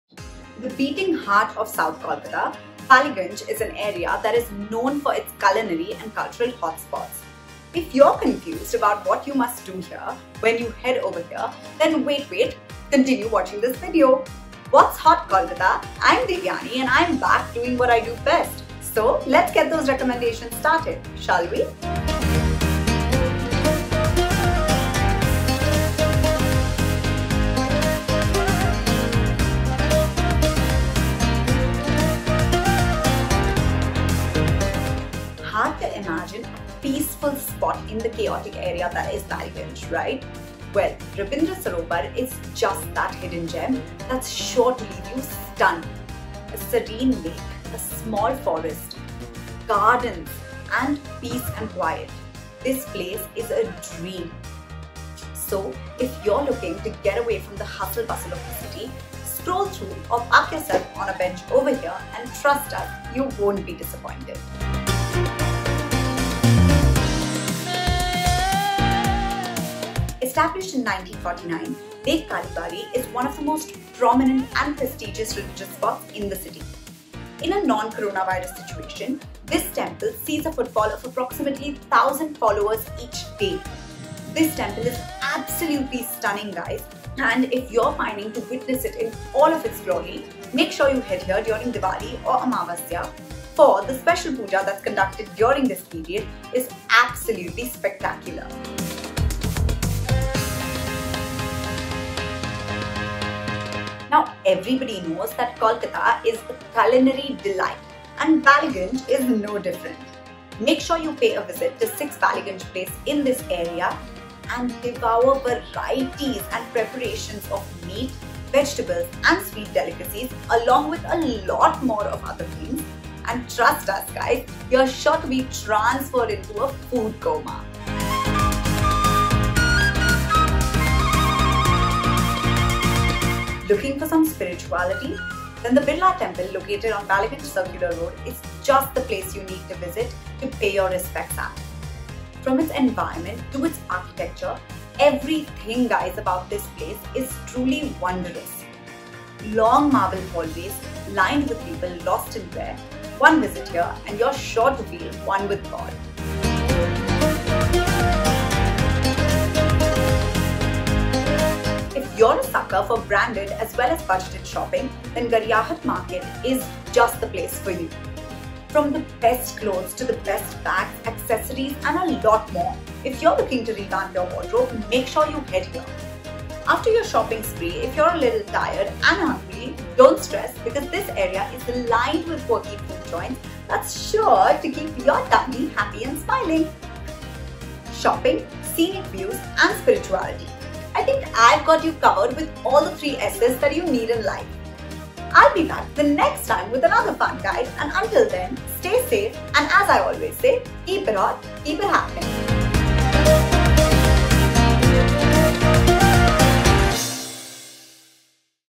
Best Things To Do In Ballygunge, Kolkata
We're back as your neighbourhood guide to show you around some fun things to do in Ballygunge!

So, what are you waiting for? Let the fun begin cos 'dil maange more'!